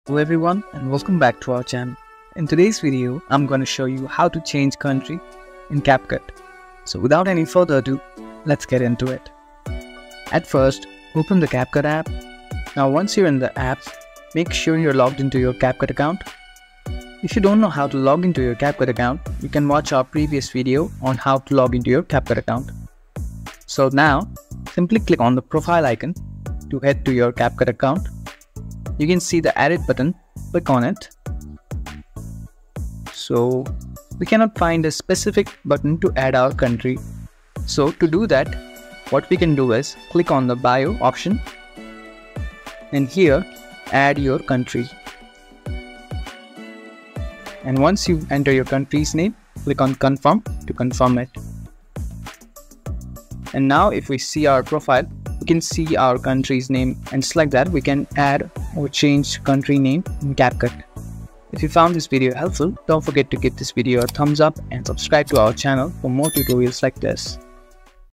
How to Change Country in Capcut 2024
Need to update your country settings in CapCut? Whether you're traveling or need to access region-specific features, this tutorial will show you how to change your country settings in CapCut easily. Follow these steps to ensure you're using the app with the correct regional settings.